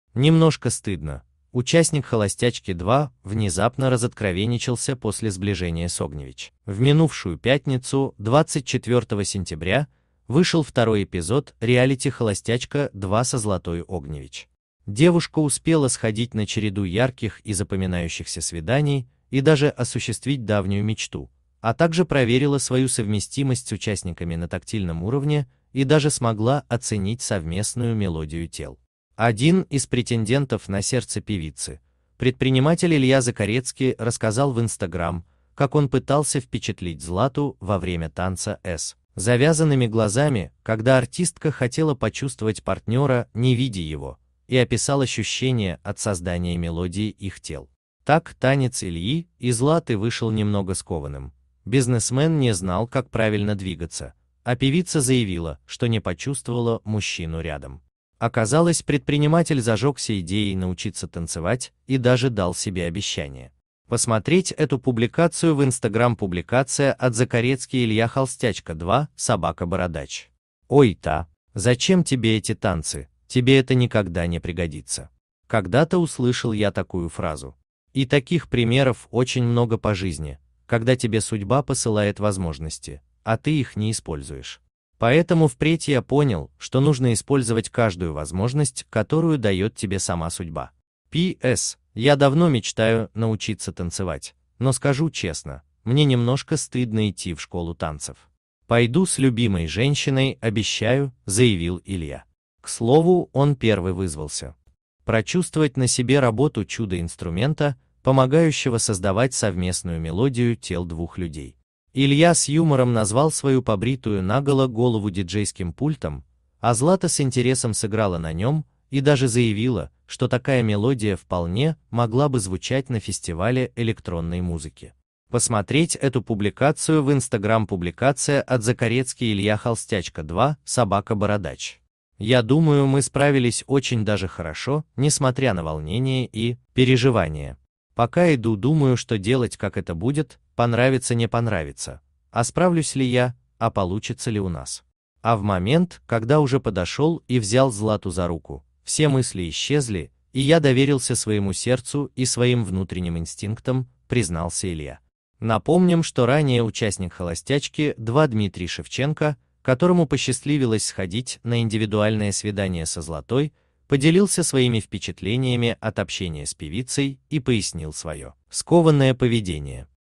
Немножко стыдно: участник "Холостячки 2" внезапно разоткровенничался после сближения с Огневич.
Холостячка ЗлатаОгневич В минувшую пятницу, 24 сентября, вышел второй эпизод реалити "Холостячка 2" со Златой Огневич. Девушка успела сходить на череду ярких и запоминающихся свиданий и даже осуществить давнюю мечту, а также проверила свою совместимость с участниками на тактильном уровне и даже смогла оценить совместную "мелодию тел". Один из претендентов на сердце певицы, предприниматель Илья Закорецкий рассказал в Instagram, как он пытался впечатлить Злату во время танца с завязанными глазами, когда артистка хотела по